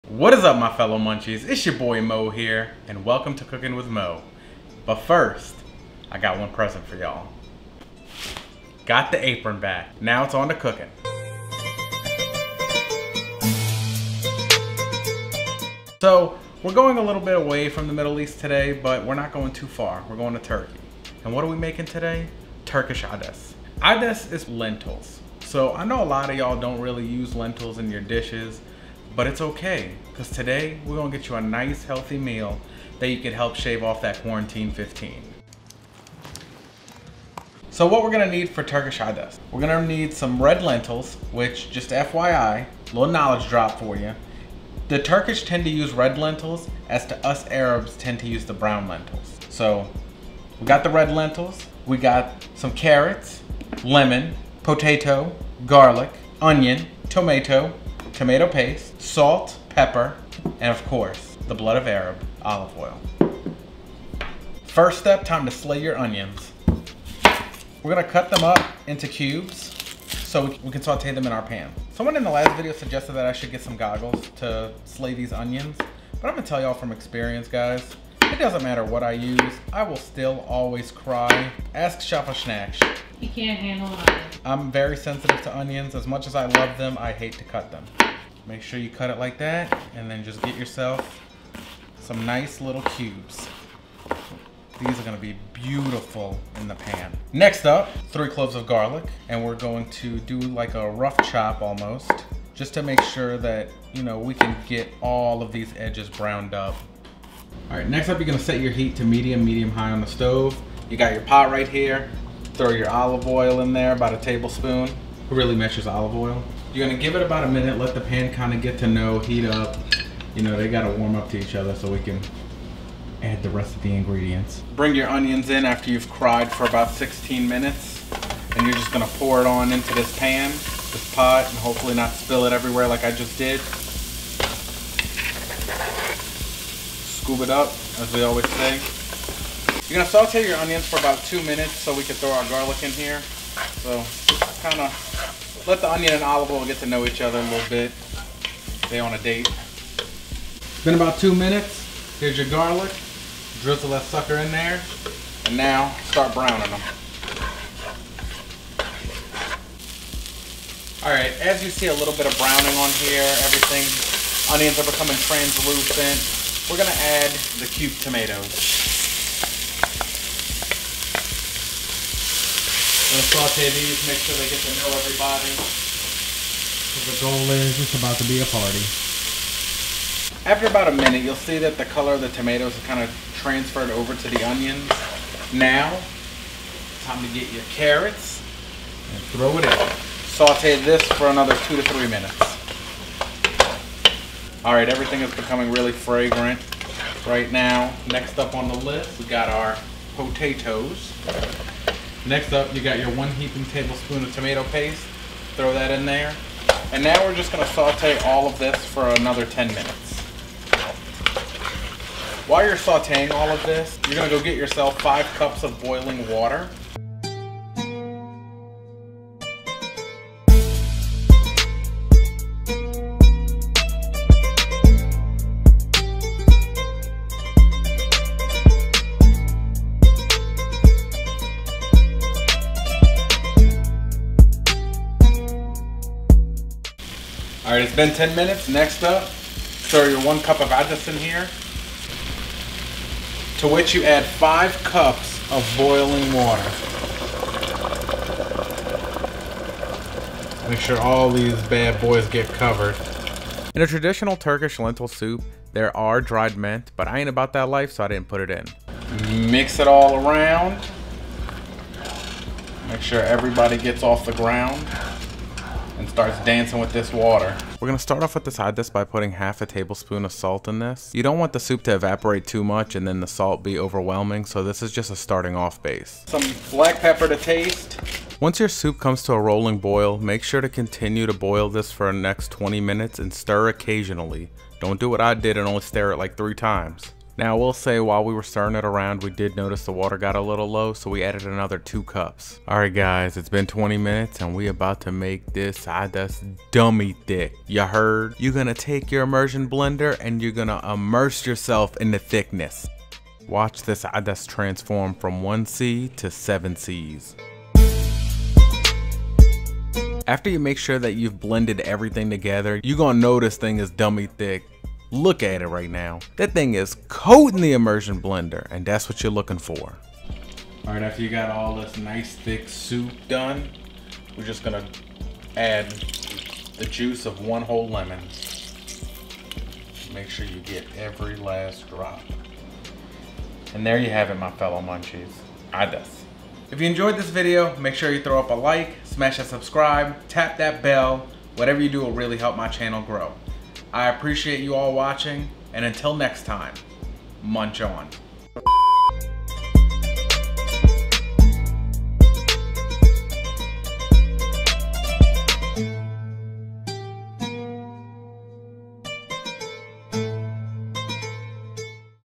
How to make a Healthy Turkish Red Lentil Soup | Vegan | Cooking with Mo Munchies
Easy Turkish Red Lentil soup is one of the best known among the Turkish Soups. It is so easy to make and very healthy and delicious.

+Turkish Red Lentil Soup:
2 medium-size onion
1 tbsp olive oil
2 medium-size carrots,
1 large russet potato
1 cups red lentils (washed)
1tbsp dried mint (tradition, but not included in this recipe)
3 cloves of chopped garlic
7-10 cups boiling water (Enough for the soup consistency)

+Garnish
Parsley